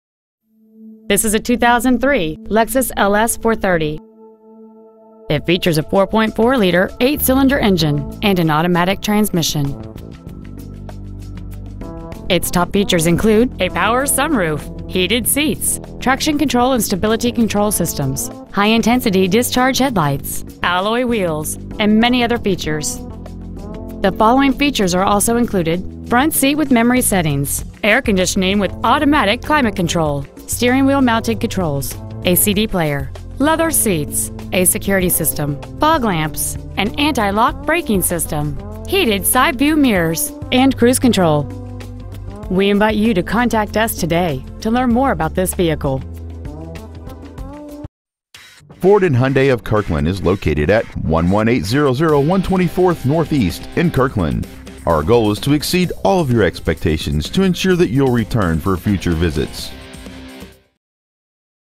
2003 Lexus LS430 Kirkland WA 98034
Year : 2003
 Make : Lexus
 Model : LS430
 Engine : 4.3L V8
 Trans . : Automatic
 Exterior : White
 Miles : 118,834
 Interior : Gray
 Stock : 385582A